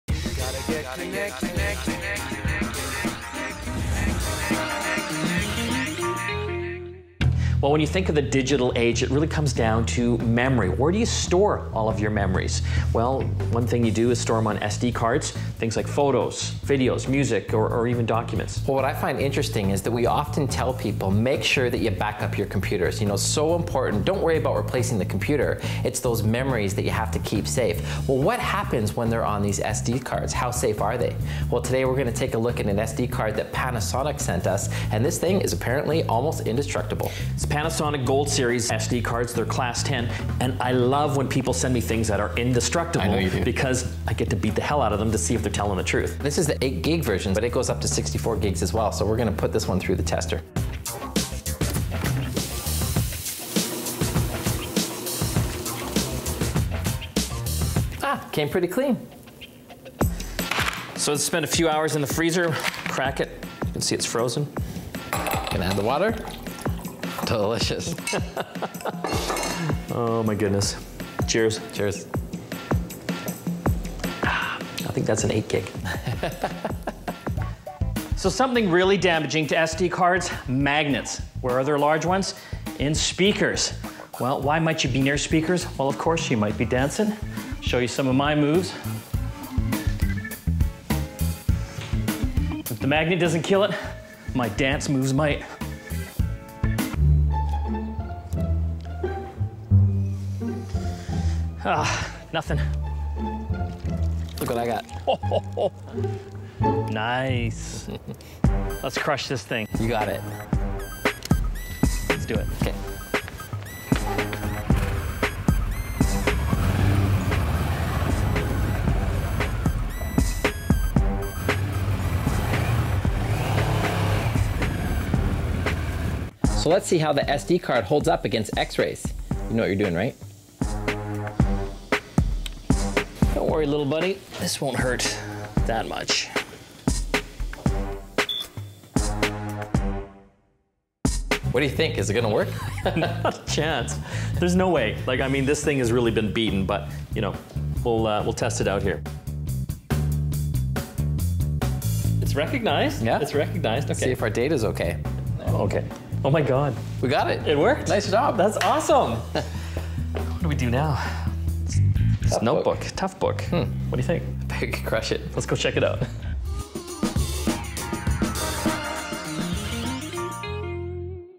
Testing The Durability of the Panasonic Gold SD Cards
Panasonic claims their Gold SD Cards are basically indestructible. We put the Panasonic Gold 8GB SD Card to the test to see if it can survive water, the freezer, magnets, being crushed and even X-Rays at the dentist. Watch to find out if the Gold SD Card survives.